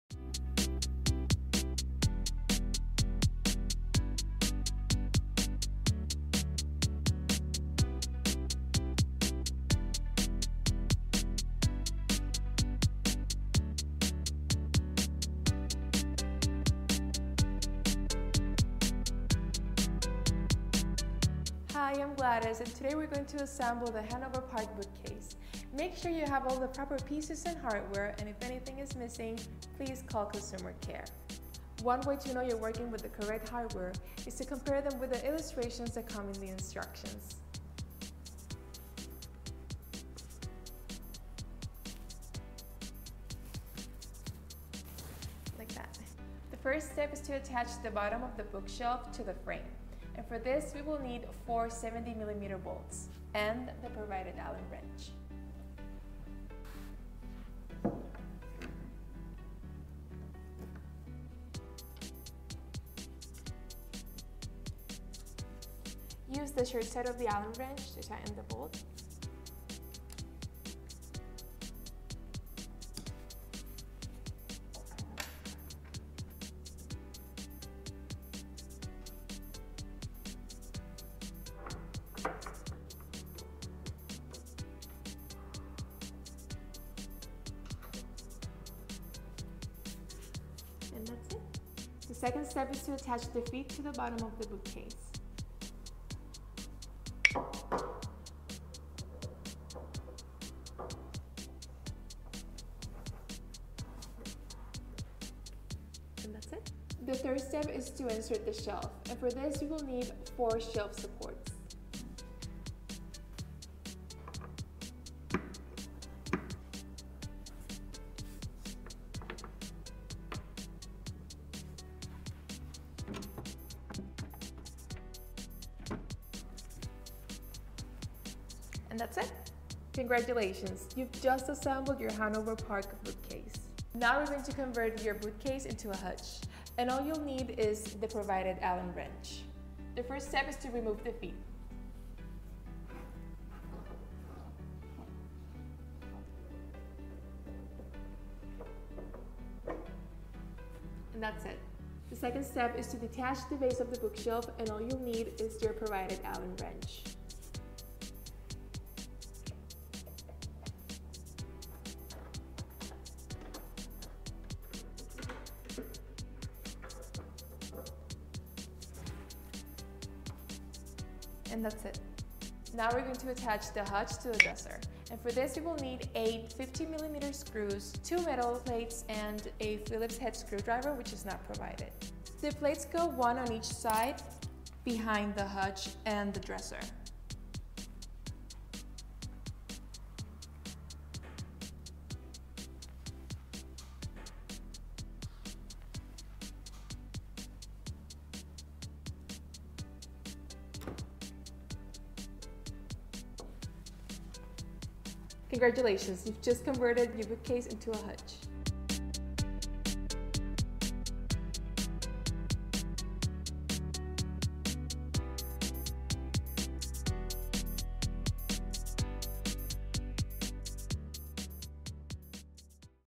Simmons Kids® Hanover Park & High Point Convertible Bookcase Assembly Video
The Simmons Kids® Hanover Park & High Point Convertible Bookcase Assembly Video works for the styles 315200 and 305200.

Here at Simmons Kids®, we are continually working to improve our assembly instruction booklets. The pictures and steps in your booklet may be slightly different than those in this video.

Use this video in conjunction with your instruction manual for the best results.

If any parts are missing or damaged, please call our Consumer Care Team at: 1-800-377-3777 Ext. 6221.